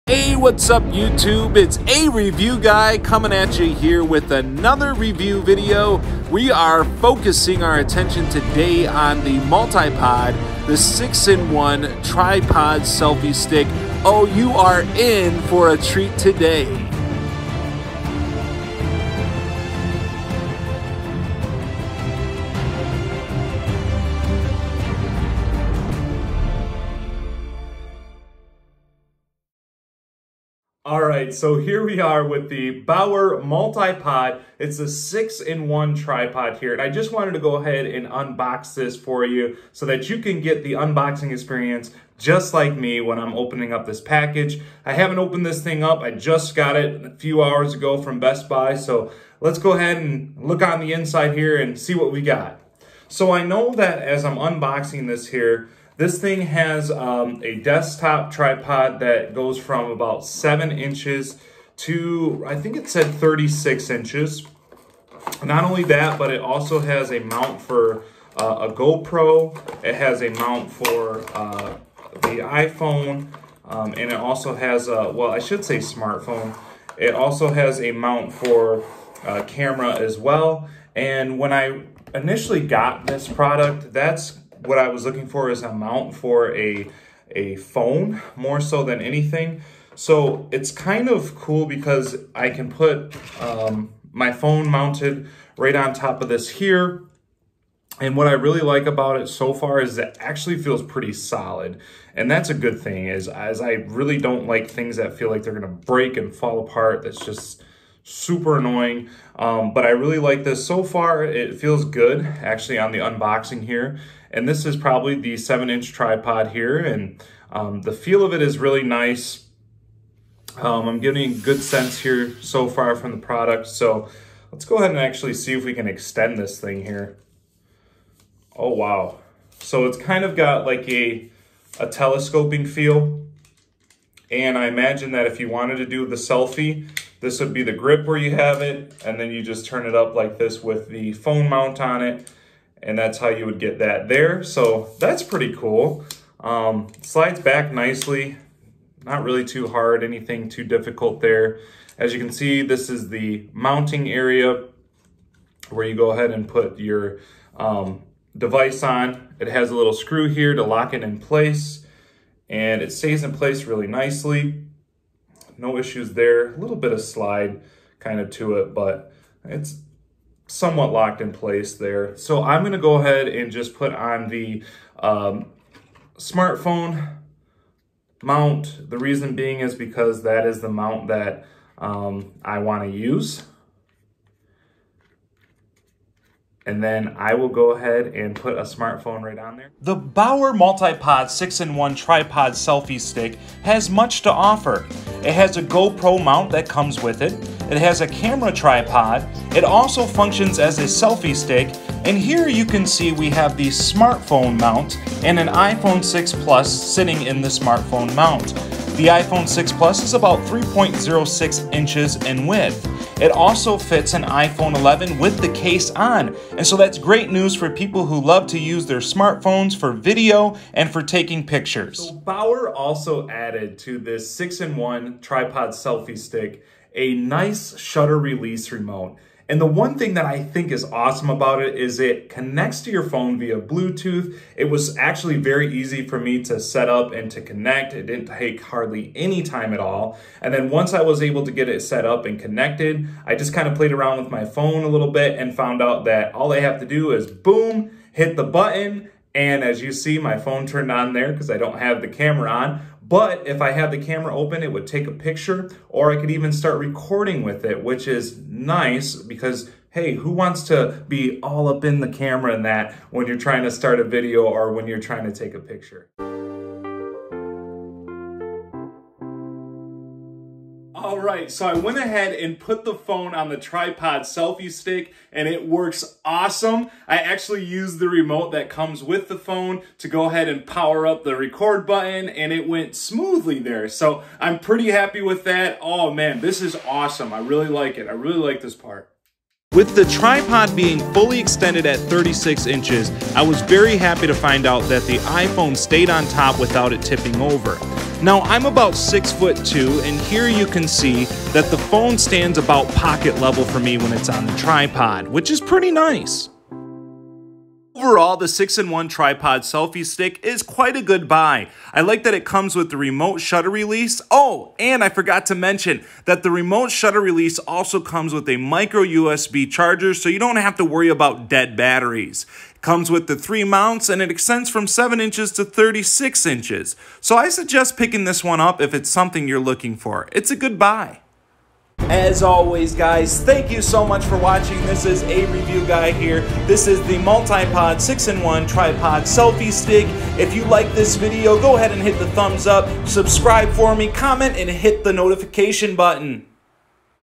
Bower Multi Pod 6-in-1 Tripod Selfie Stick Review l Unboxing
Bower Multi Pod 6-in-1 Tripod Selfie Stick is an inside look on a relatively inexpensive and useful tripod. It can be used for taking pictures, filming and getting that stable shot! Watch the video to find out more.